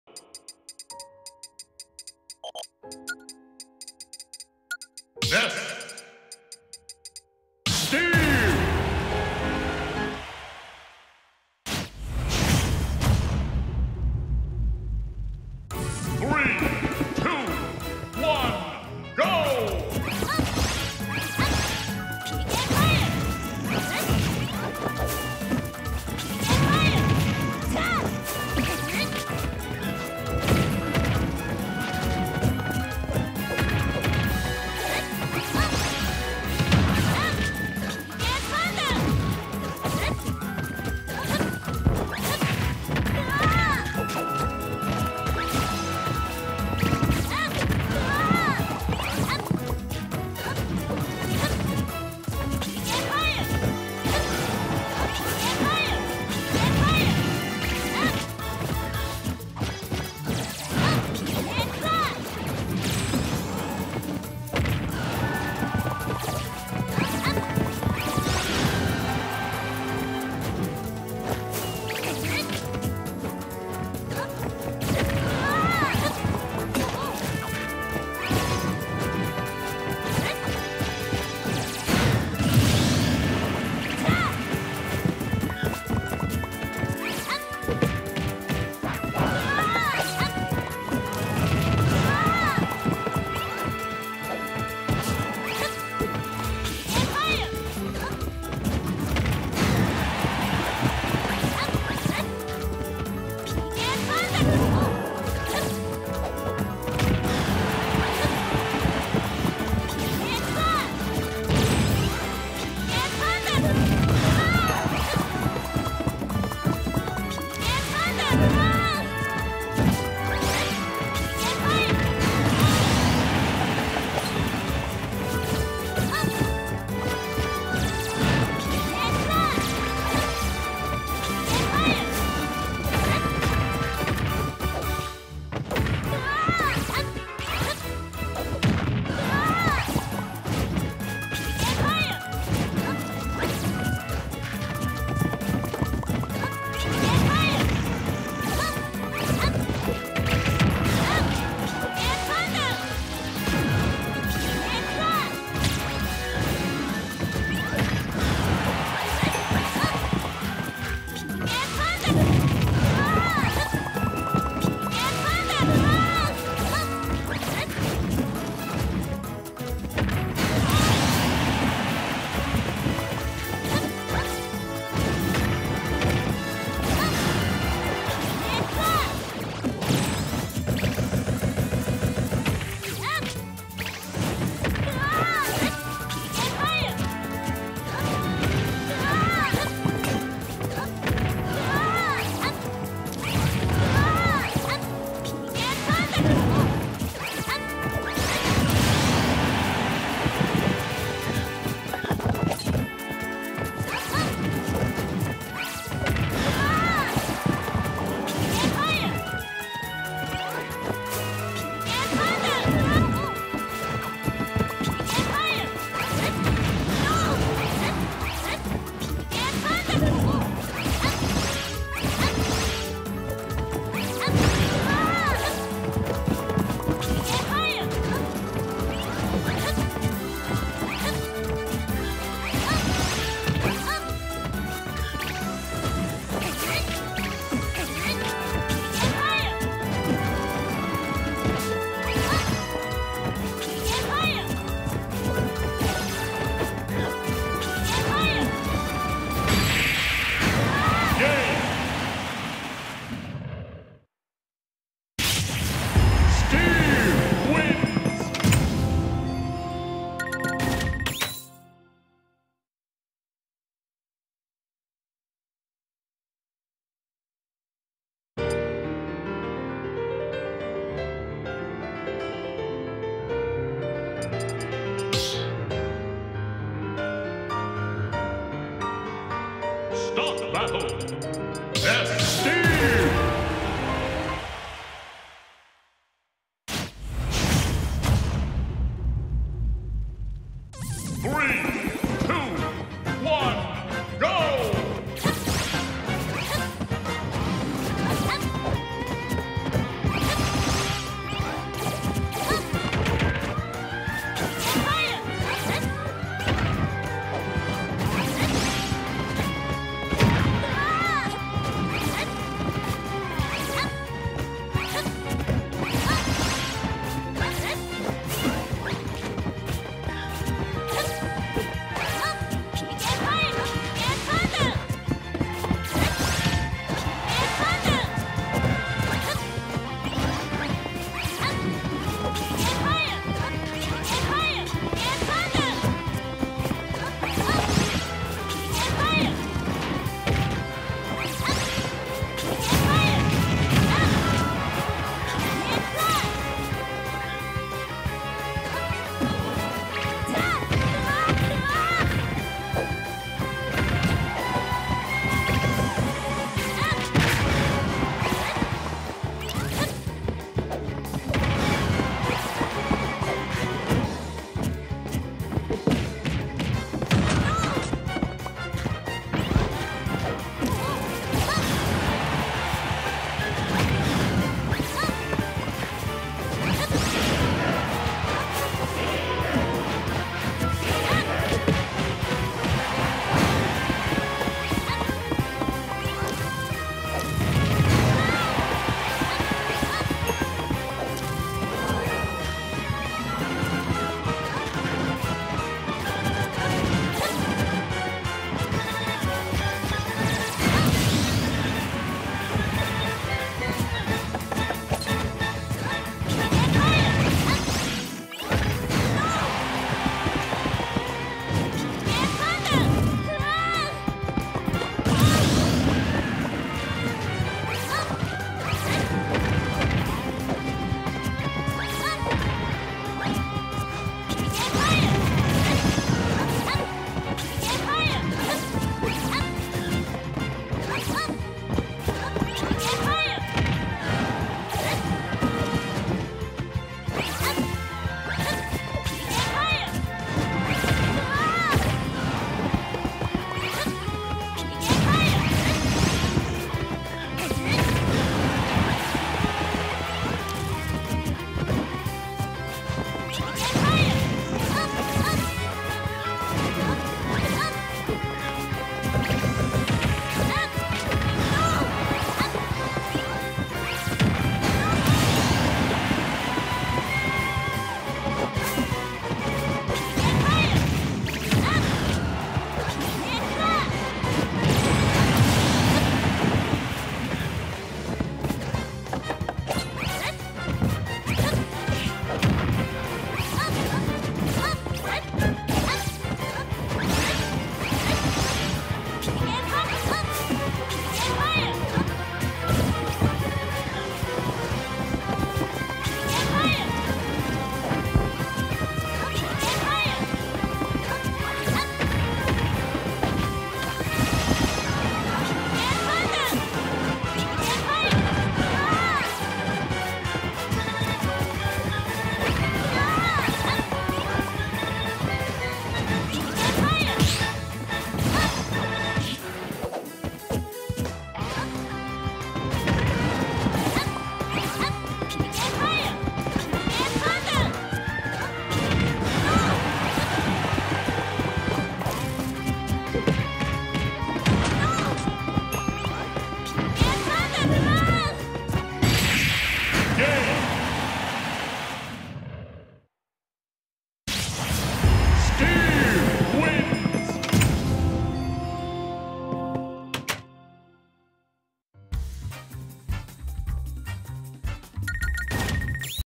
FNF #6 Jake Fox (Ness) vs HeroAwesome (Steve) Losers Top 8
Took place at Section 9 in Fargo, North Dakota. Interested in coming out?